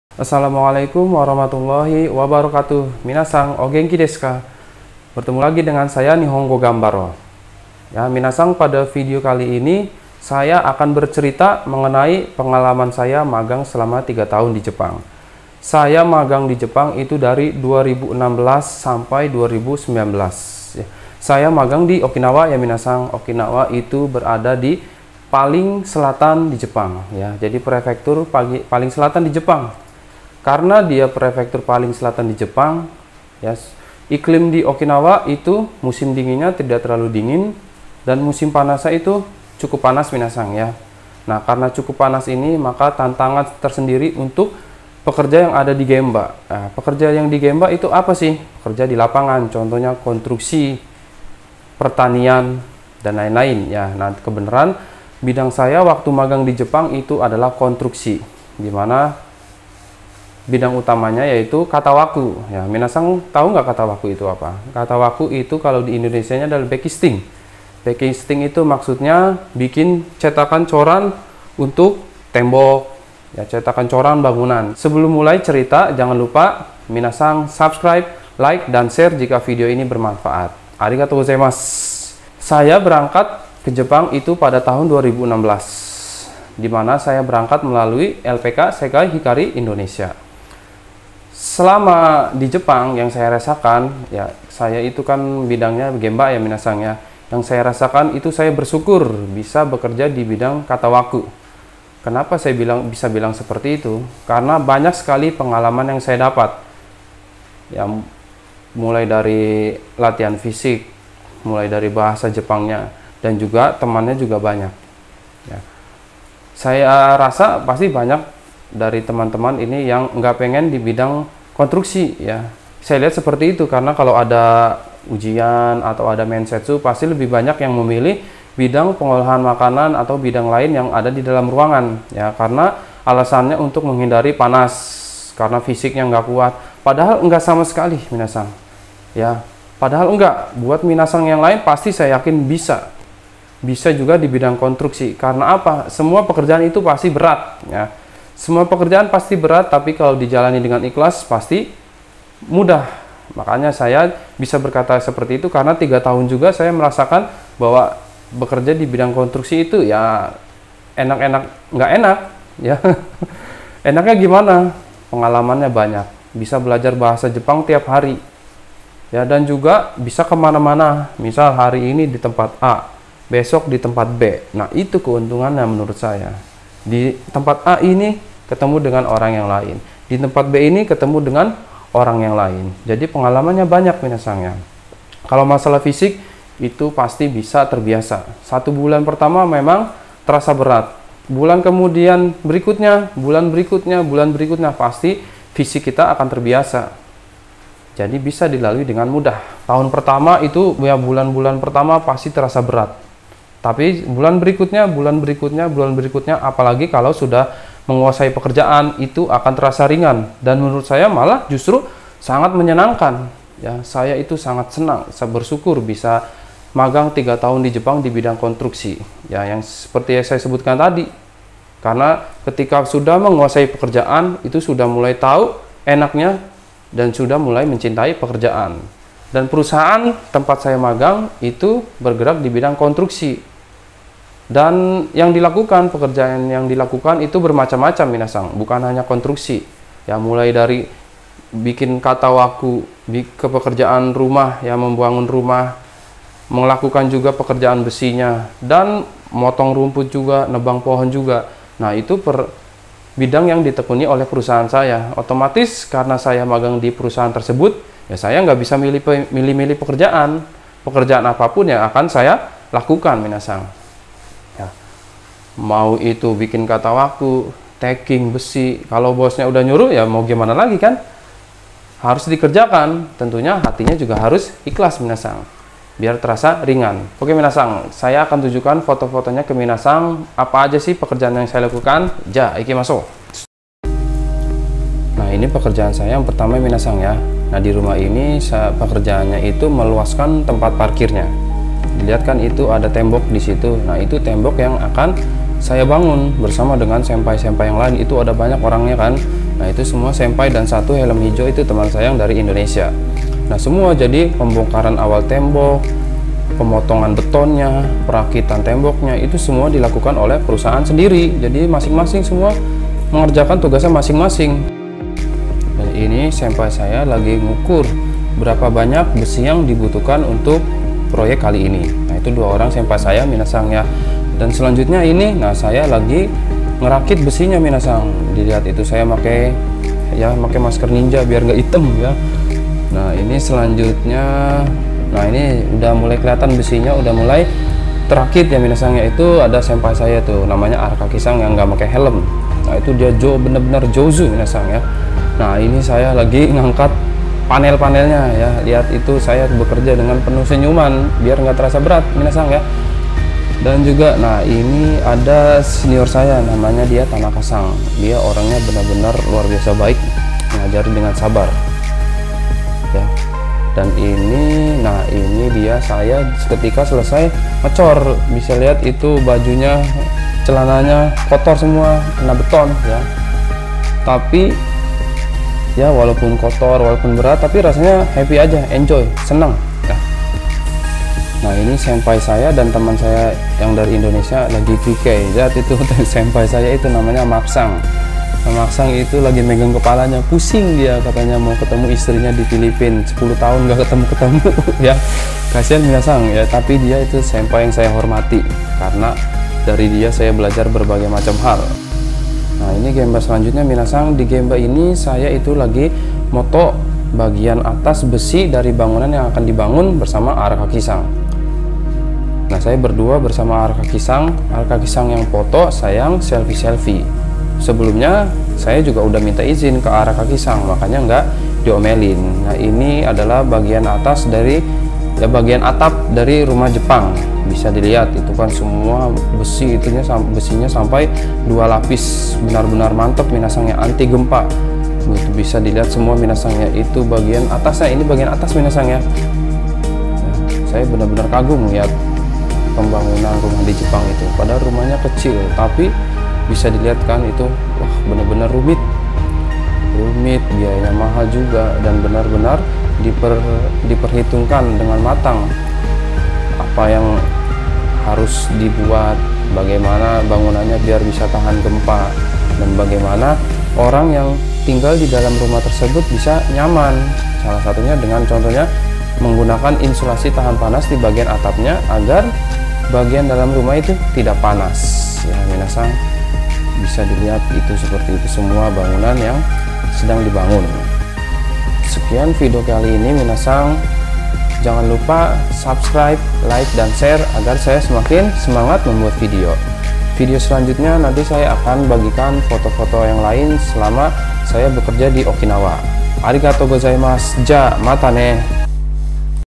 Bagaimana rasanya bekerja dijepang bidang konstruksi?
Di video ini saya akan share mengenai bagaimana rasanya bekerja dibidang konstruksi jepang dan pekerjaan apa saja yang sudah saya lakukan

magang jepang, kerja dijepang, konstruksi jepang, katawaku jepang